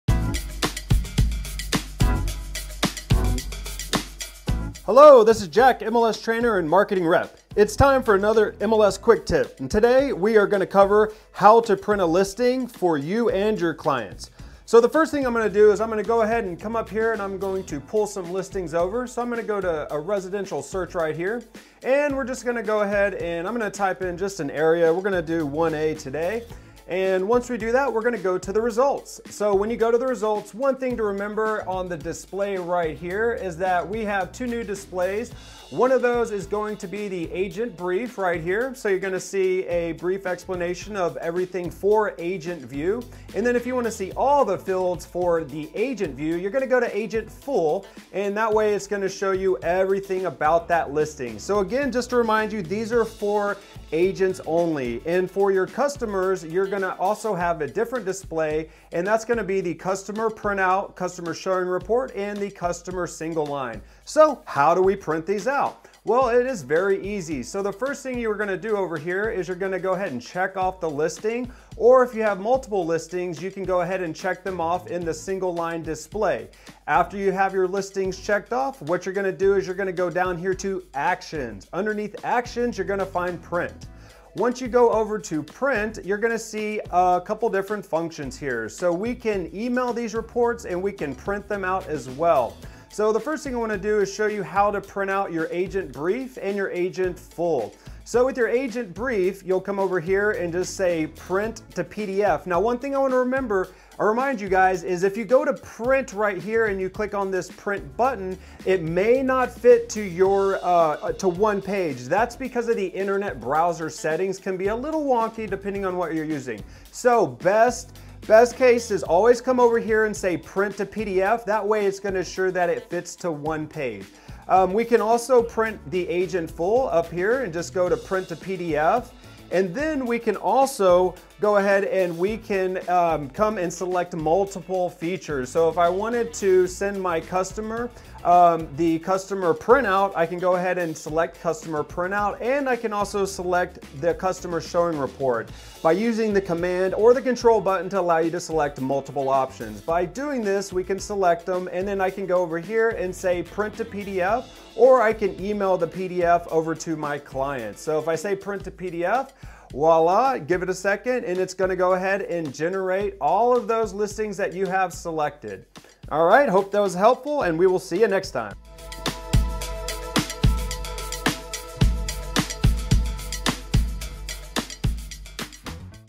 MLS Quick Tip | Printing a Listing
Have questions about printing listings? Follow along as we show you how to quickly print the Agent Full and Agent Brief pages, including how to email a listing in Matrix.